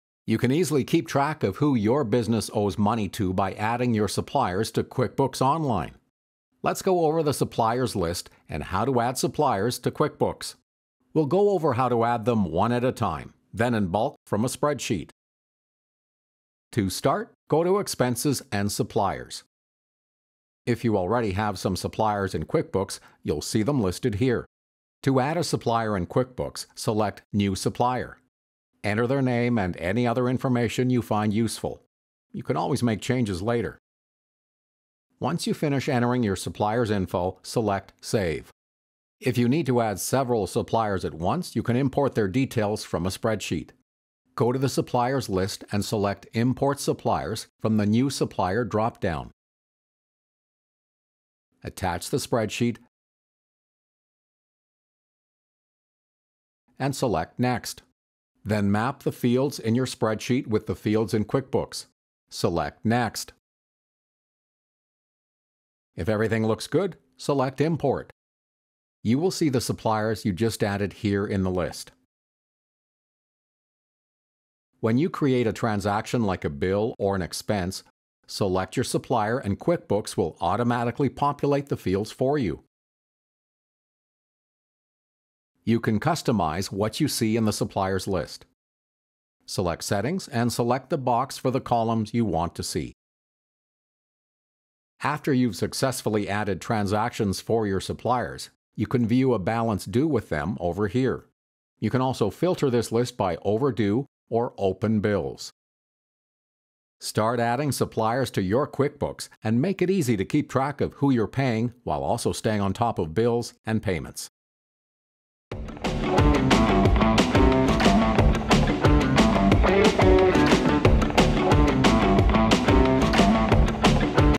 How to add suppliers
Learn how to add suppliers either one by one or in bulk using a spreadsheet.